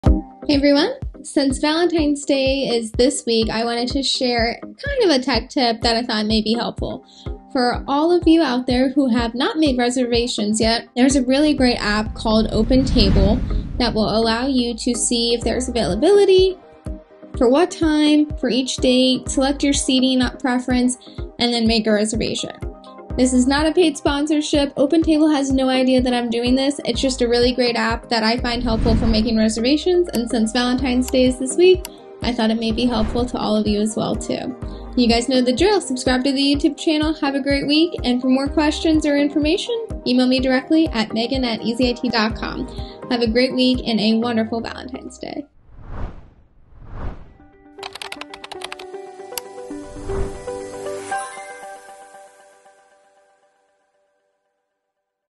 Open Table Helps You Stay In The Good Books This Valentine's Day
With Valentine’s Day just around the corner, many are scrambling to make last-minute plans with their loved ones. An invaluable tech tip for such occasions is utilizing OpenTable, an app crafted to streamline the reservation process. Not only does it display available time slots at your chosen destination, but it also offers customization options for seating preferences before confirming the booking.

This user-friendly app comes highly recommended, and though this isn't a paid endorsement, it has greatly assisted numerous others, making it worthwhile to share as you finalize your Valentine’s Day arrangements.

ADDRESS
4150 Tuller Road,
Suite 234